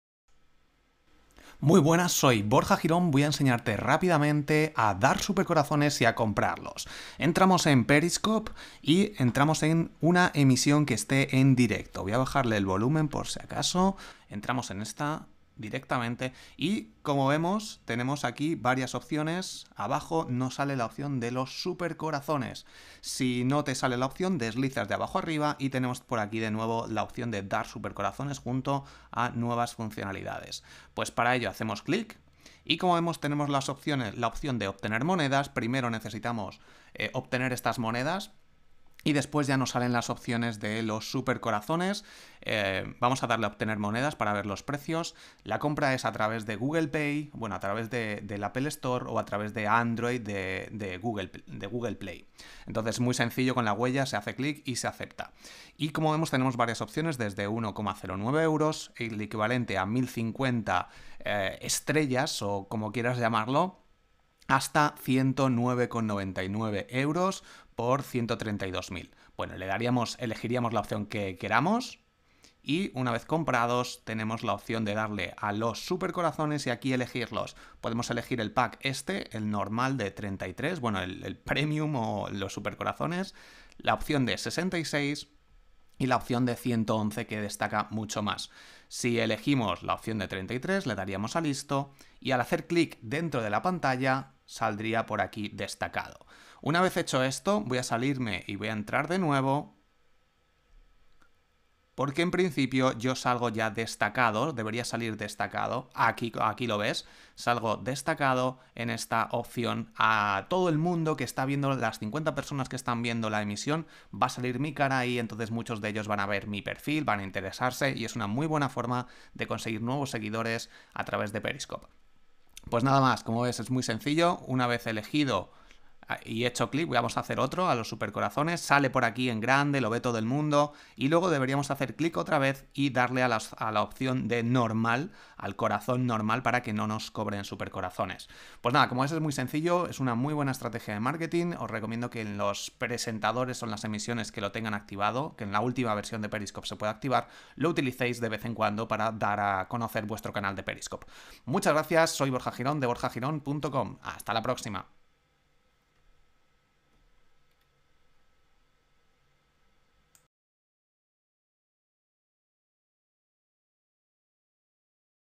▷ QUÉ SON, CÓMO COMPRAR Y DAR SUPERCORAZONES EN PERISCOPE
Los super corazones de Periscope son una nueva funcionalidad con la que puedas ganar dinero de forma directa emitiendo en Periscope.

Los espectadores compran monedas y esas monedas se transforman en supercorazones que se pueden dar a los presentadores.

El objetivo es apoyar al presentador para que siga emitiendo su contenido de forma periódica.

El 70% va al presentador y el 30% para Periscope.

En el vídeo te enseño a comprar super corazones con un iPhone (con Adroid es igual) y el pago se hace a traves del Apple Store o del Google Play por lo que está completamente integrado.

Dar supercorazones en Periscope es una muy buena estrategia de Marketing para potenciar y dar a conocer tu canal de Periscope ya que saldrás DESTACADO durante la emisión en la que des SUPERCORAZONES.

Nota: De momento no se puede cobrar a no ser que seas residente de USA. Además debes darte de alta como Super Broadcaster para dar tus datos de Paypal y recibir el pago. Alta Super Broadcaster